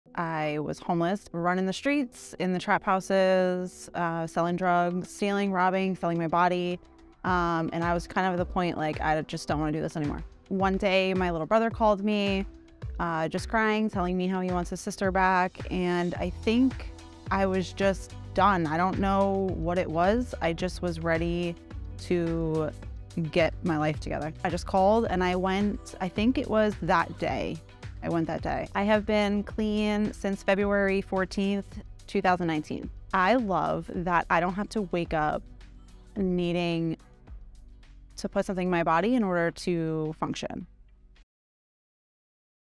I WAS HOMELESS RUNNING THE STREETS IN THE TRAP HOUSES
Ashley discusses her drug addiction and sobriety.